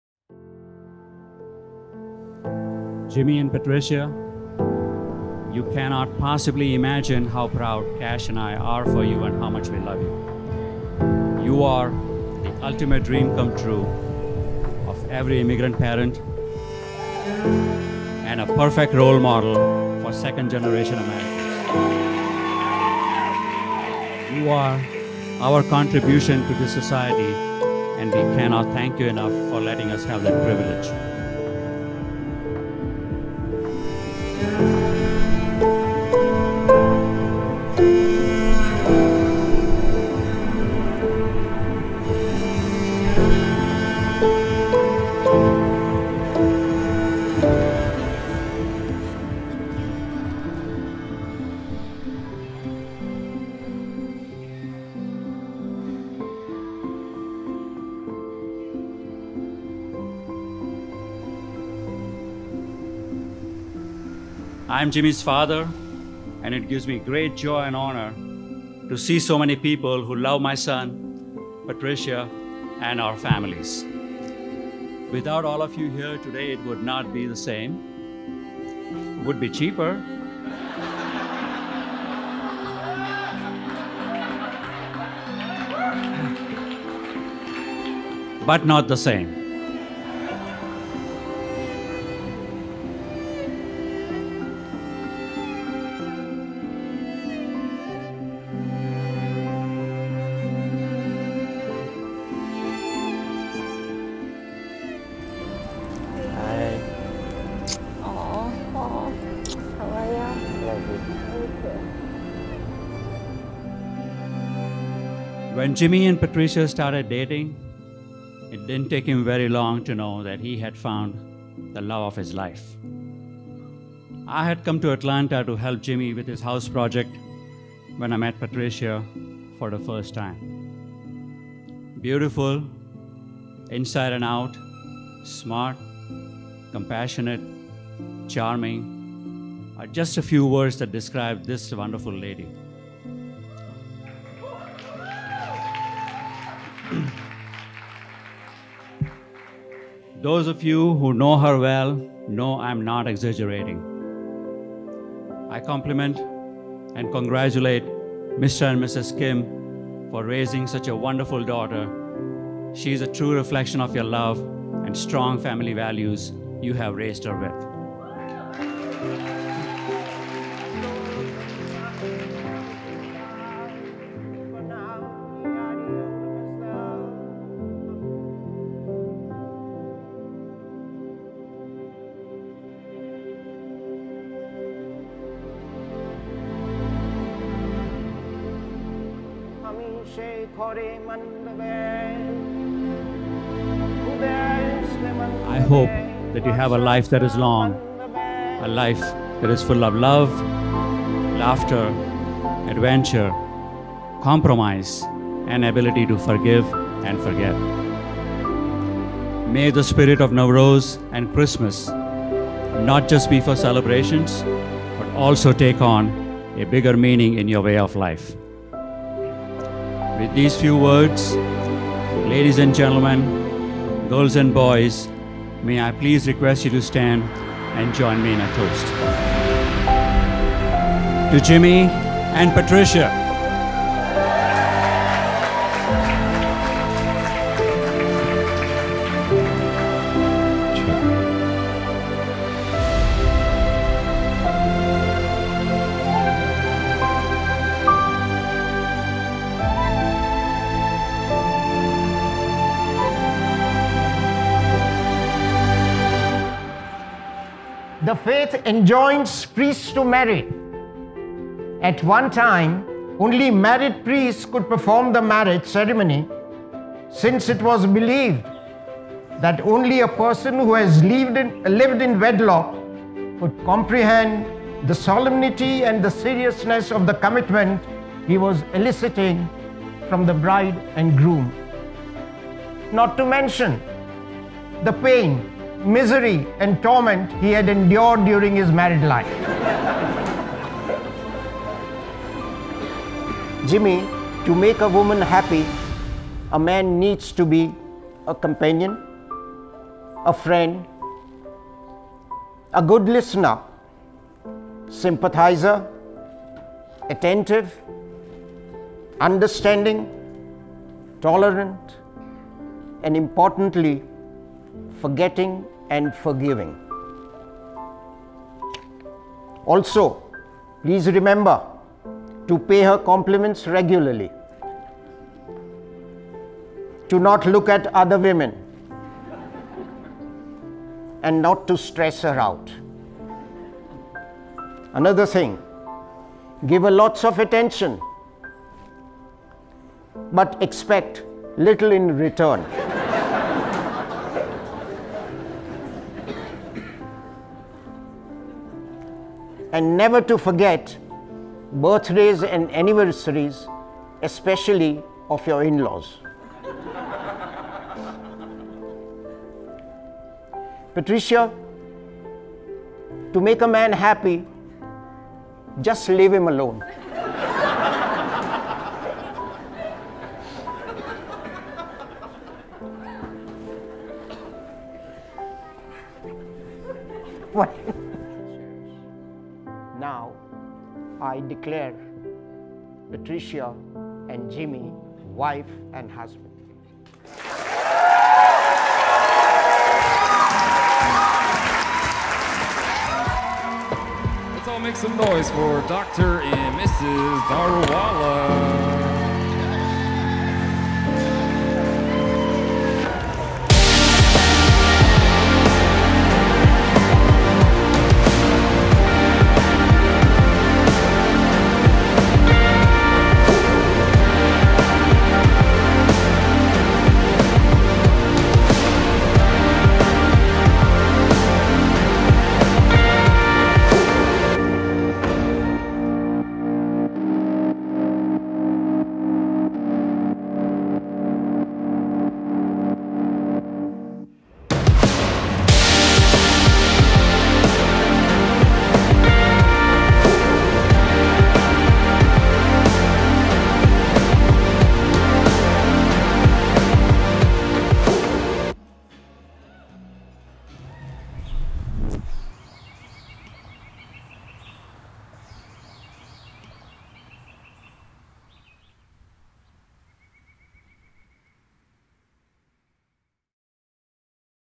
POWERFUL Speech from Father to Son | Summerour Studio in Atlanta, GA Wedding | Jimmy and Patricia
Jimmy and Patricia celebrated their beautiful wedding event at Summerour Studio in Atlanta, GA. I would love to tell their story right here, but Jimmy's father can do a much better job, just listen to how powerful his speech is to his son!